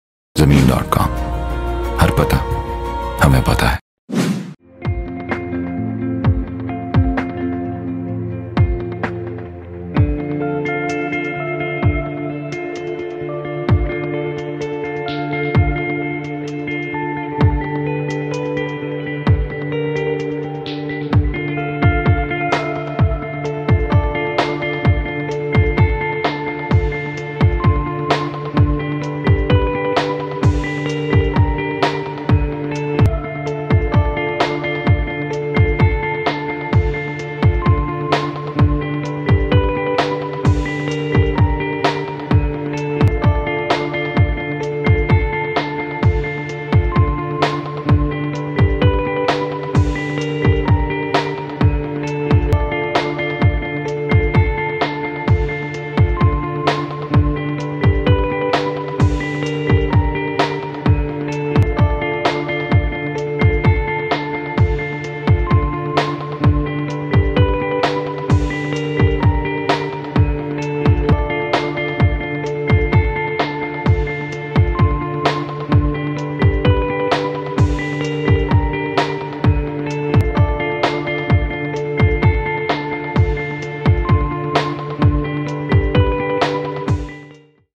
1 KANAL RESIDENTIAL PLOT FOR SALE IN CAPITAL SMART CITY ISLAMABAD
Capital Smart City Islamabad is a project Of Habib Rafiq Pvt Limited Near New Islamabad international Airport with Exclusive designated Interchange On M2 Motarway.

5 mint Drive from New Islamabad International Airport.
25 Mint drive from Islamabad zero Point.

Applicant NICOP is Required.

Corner category and Main Boulevard category is Available.
Balloting After 6 Month.